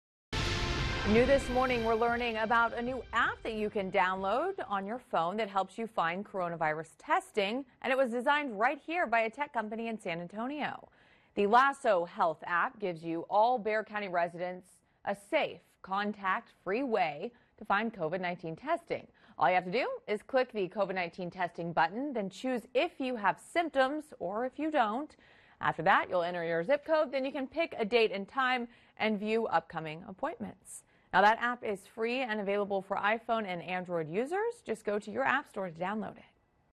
LASO HEALTH APP- NEW THIS MORNING/ FOX29
Download the free LASO app from Apple or Google Play Stores and book your testing appointment today. If you have symptoms or have been exposed to someone who has tested positive you can book a no cost appointment through the LASO mobile app to speak to a licensed Physician and receive a COVID-19 test if necessary.